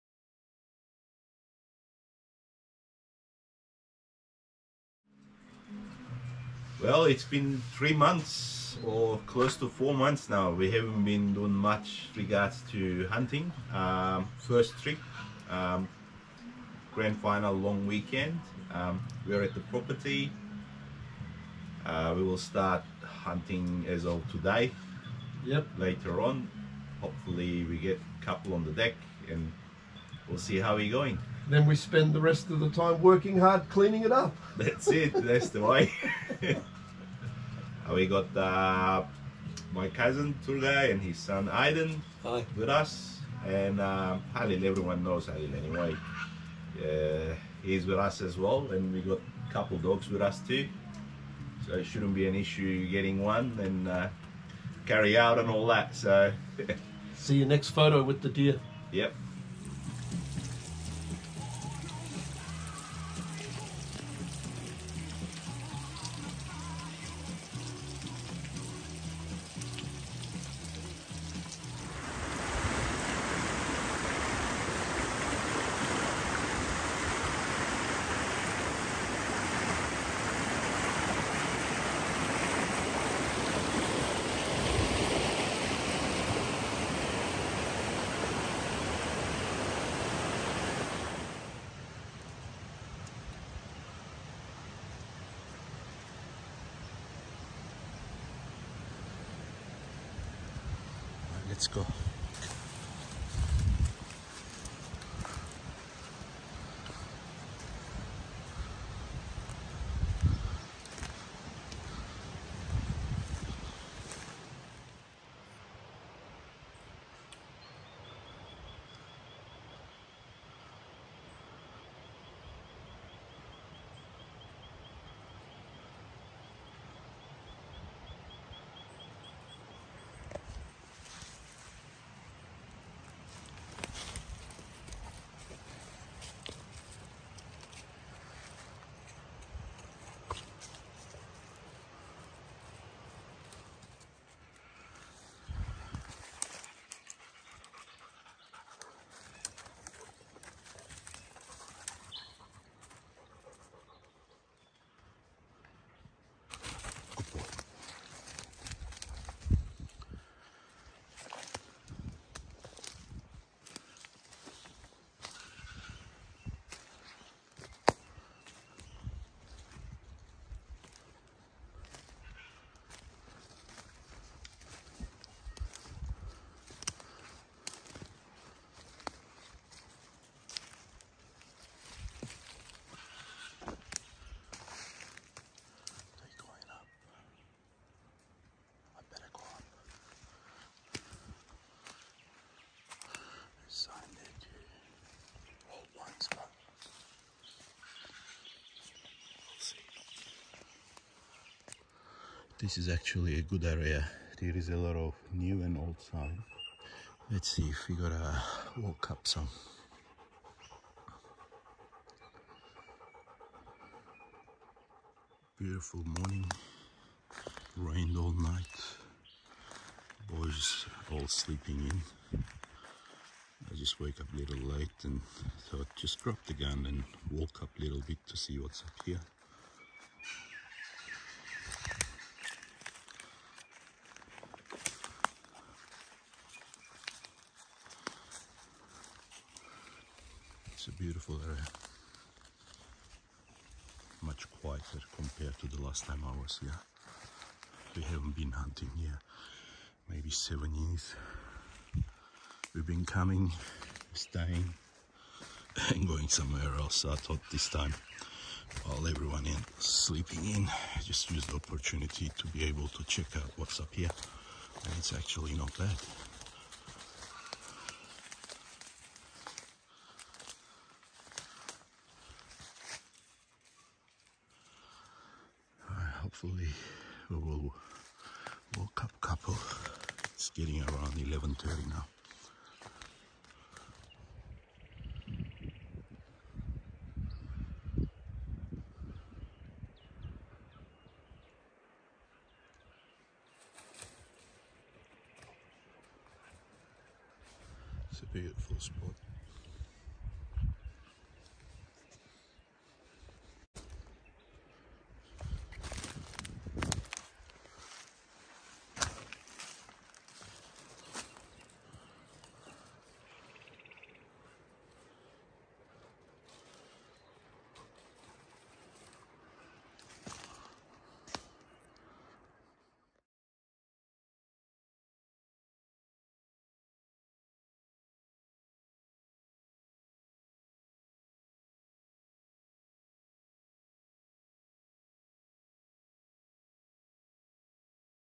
Sambar Deer Hunt - 2022 Grand Final Weekend
First hunting trip in 4 months due to busy work schedule , great times in the bush .